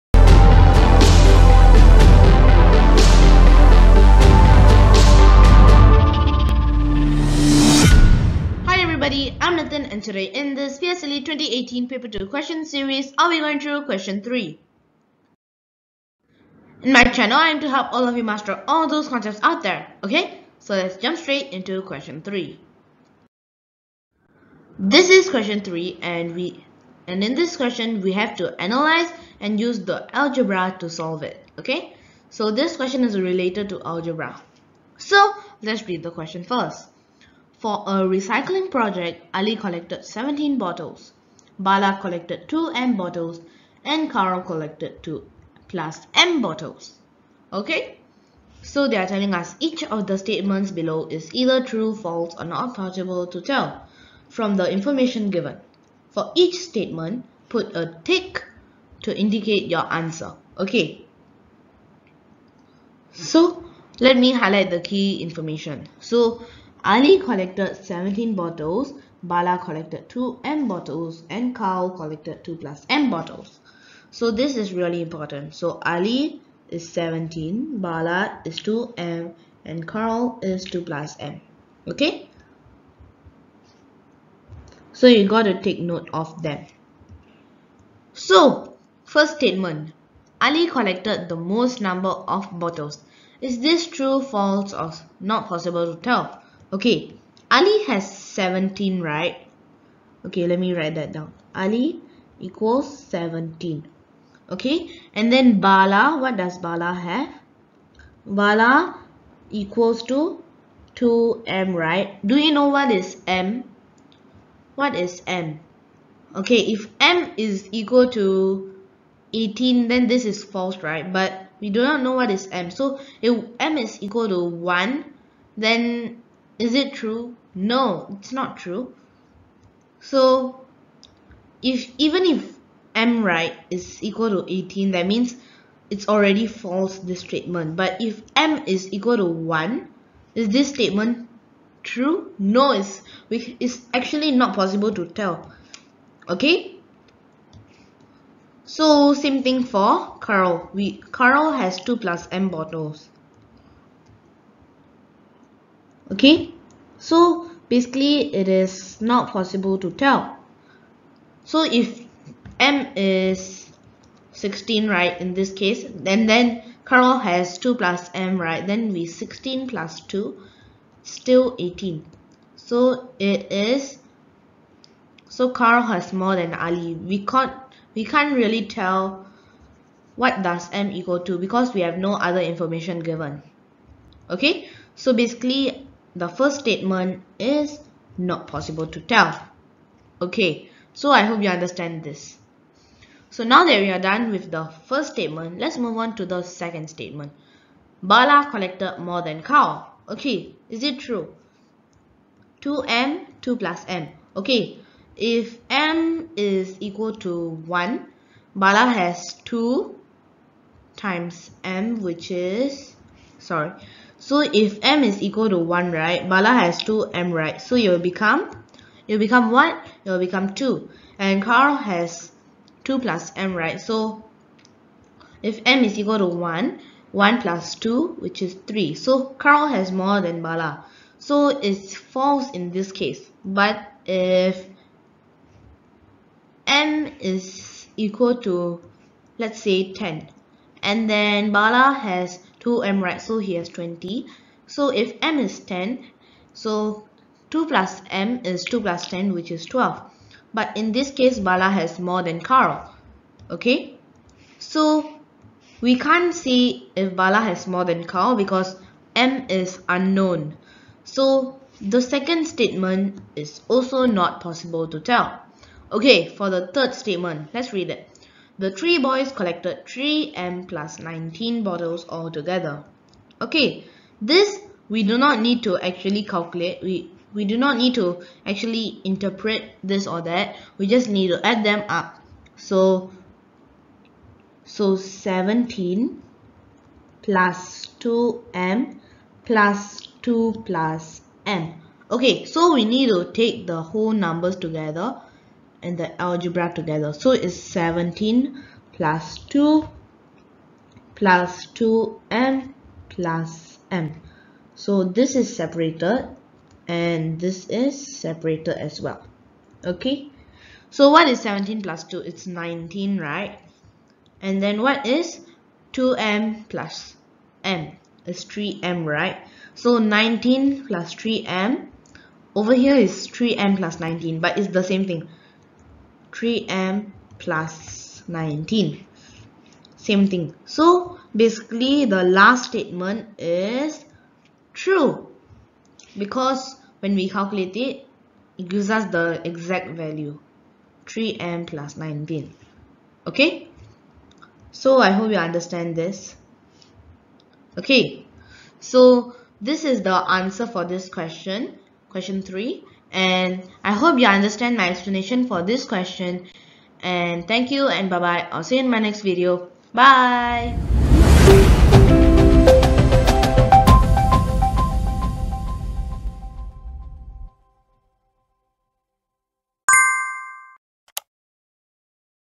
PSLE Math 2018 | Paper 2 Question 3 | Primary 6 Singapore | Analysis & Algebra
About this Video: Question 3 analysis and algebra
The video is about solution to the question from the section of algebra in mathematics. Algebra which is a part of mathematics basically is all about specific varying values that are assigned to alphabets that are used as the reference symbols. In the video the solving of the question is carried out along with the analysis of the methodical logic the tis applied while solving the problem. The question is explained and the method described in a simple and easy way.